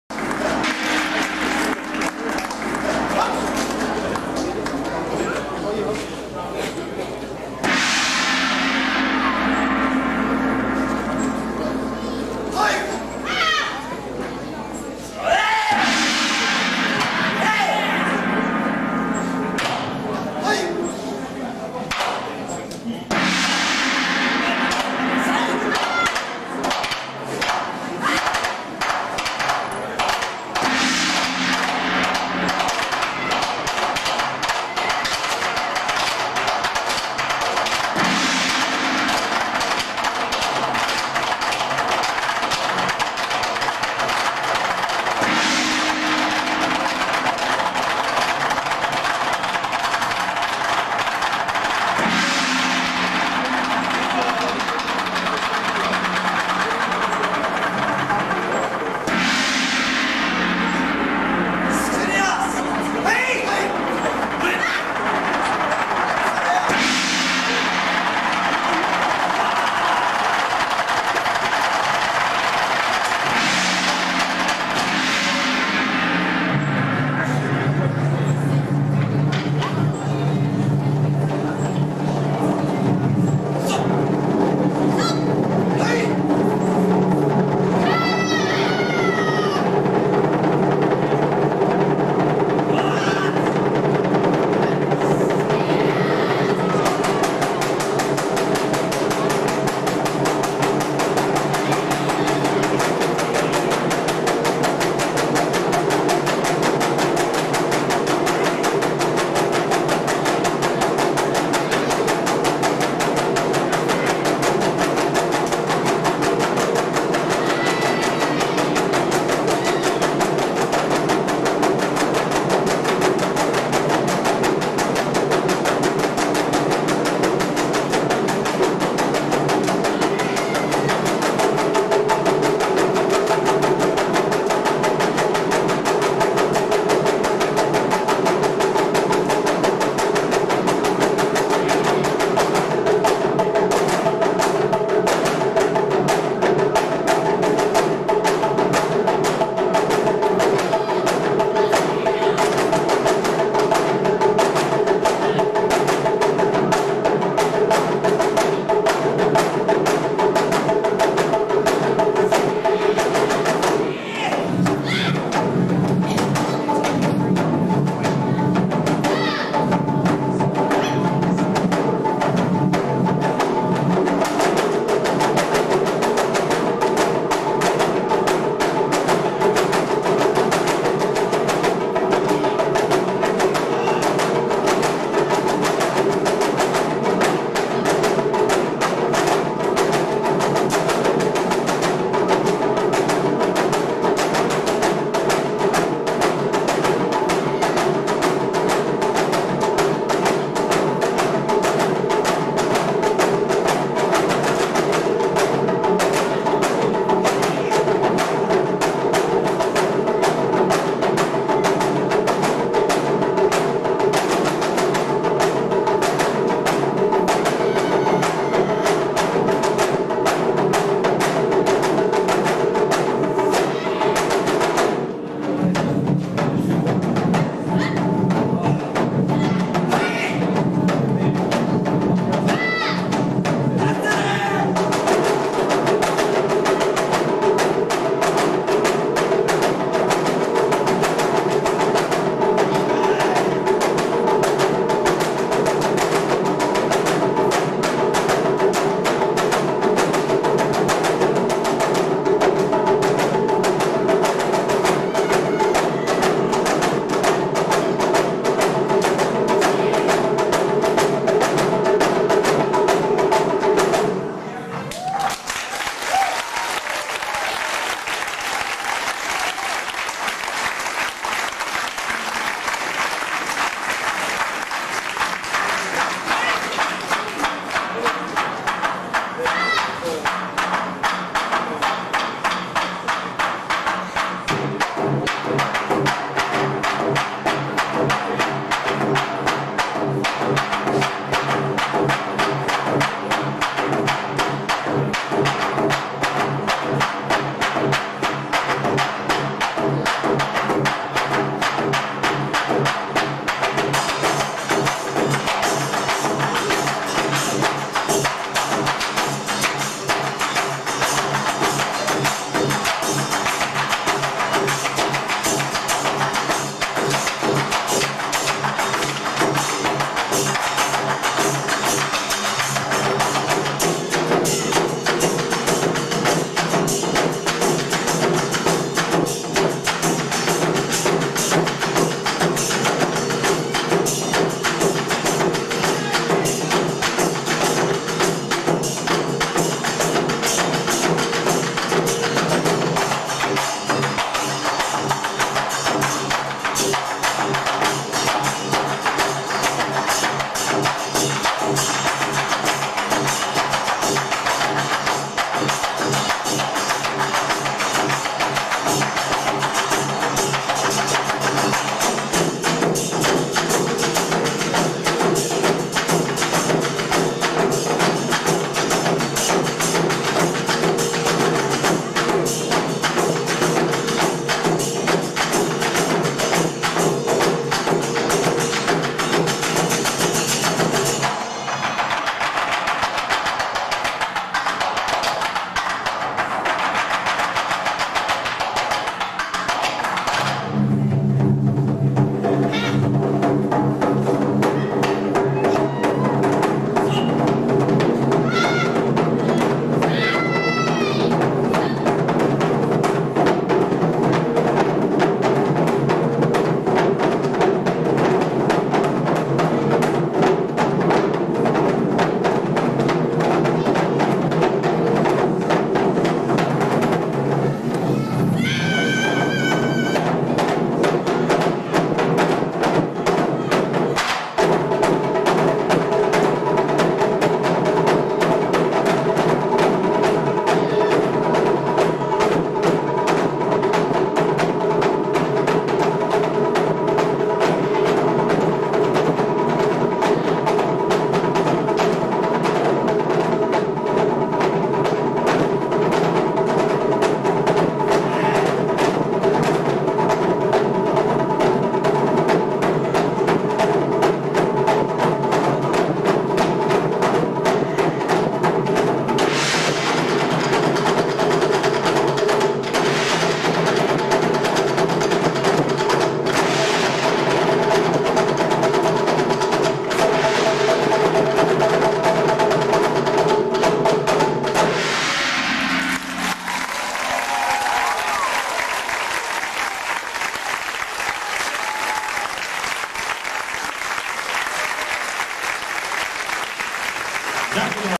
Yakudo 1/3
Japanese Drumming (Taiko) group from Toronto, Canada during the annual Caravan Festival. Clip 1 of 3.